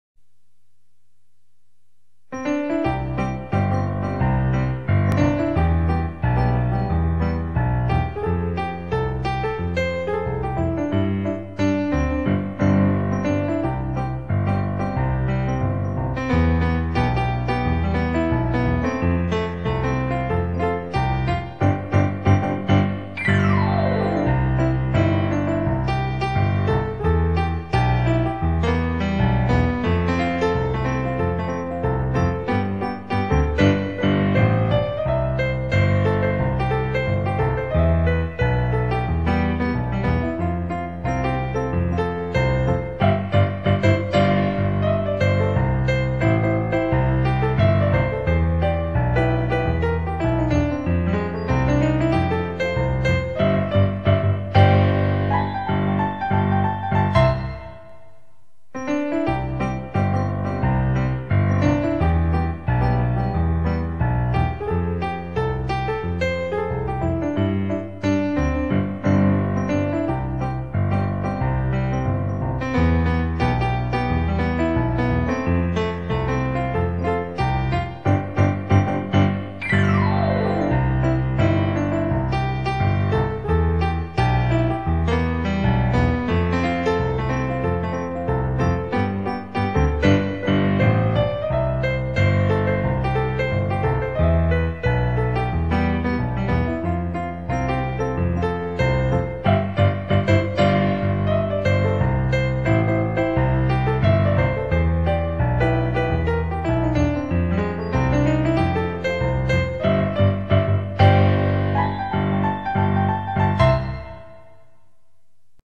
Relaxing music - Jazz -- funfair, enjoy,happy mood....
*Homemade music@
QUALITY OF LIFE STARTING FROM RELAXING STRESS.
Sound of peace from Heart , language of naturals , relaxing melody , a gift from creator.
Original music composed by : Chan Jun Mon
Performed by : Chan Jun Mon
Instrument : YAMAHA DGX 640